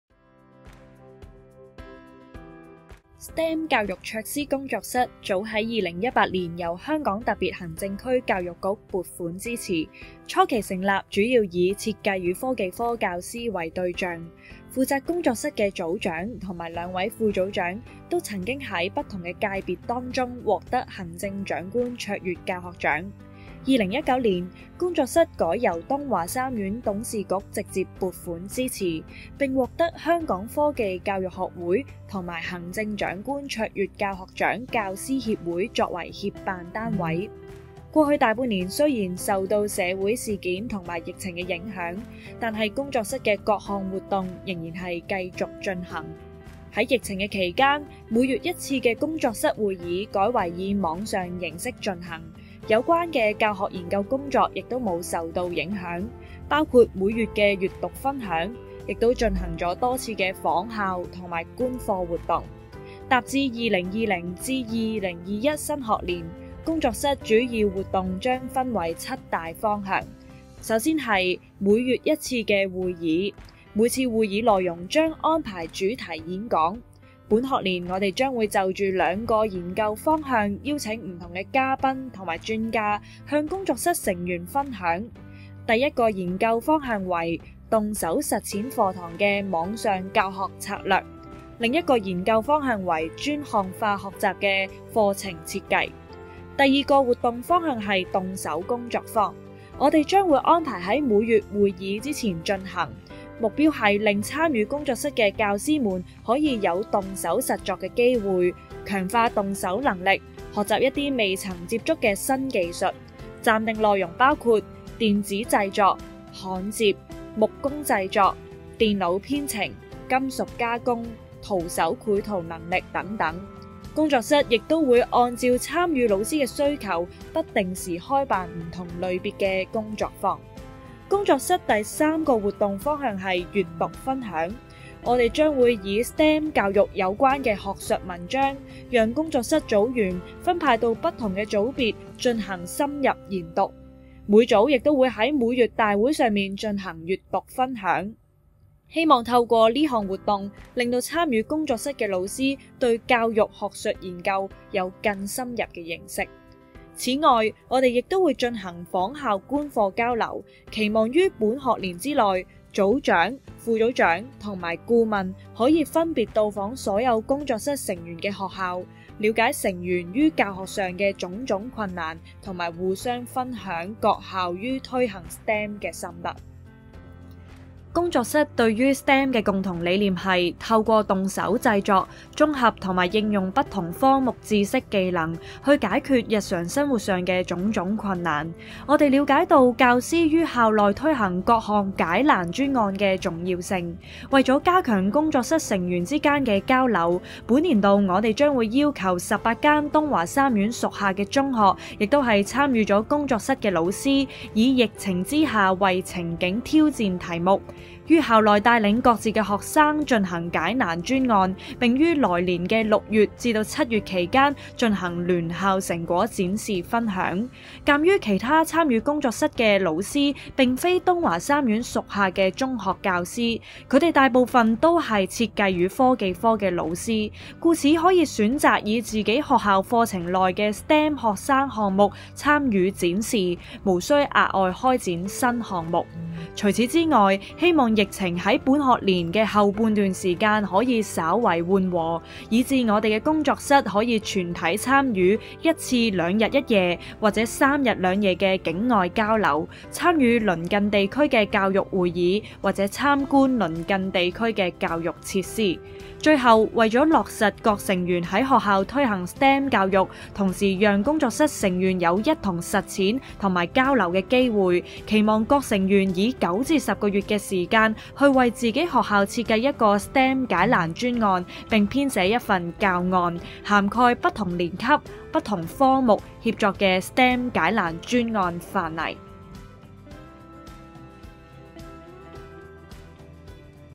【卓師工作室】 STEM教育 卓師工作室 2020-2021
卡洛斯作為音樂範疇的卓越教師得主，竟然獲邀參加科技教師的卓師工作室，還是感謝梁偉業老師俾面！